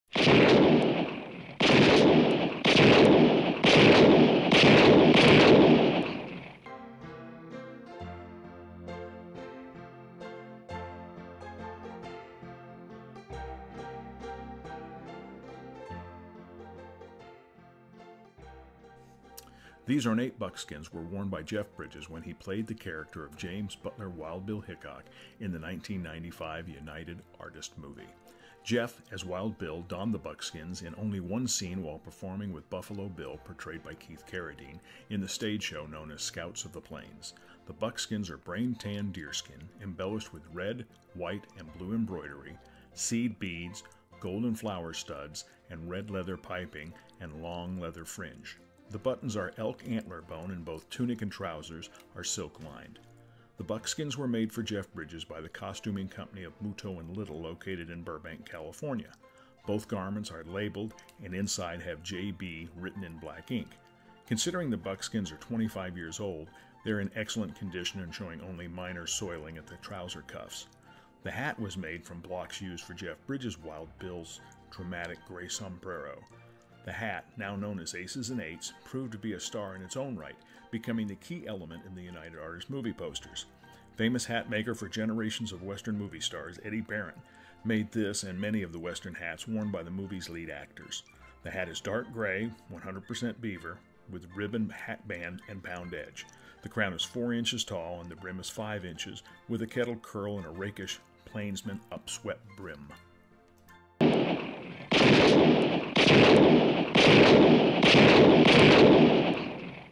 Wild Bill's Buckskins
These ornate buckskins were worn by Jeff Bridges when he portrayed James Butler Wild Bill Hickok in the 1995 United Artist movie "WILD BILL". The detail and craftsmanship are remarkable and after 25 years they are still in like new condition.  Here is closer look at the artistry in leather fashioned by Muto and Little of Burbank, CA.
By the way, the hat’s pretty darn nice too!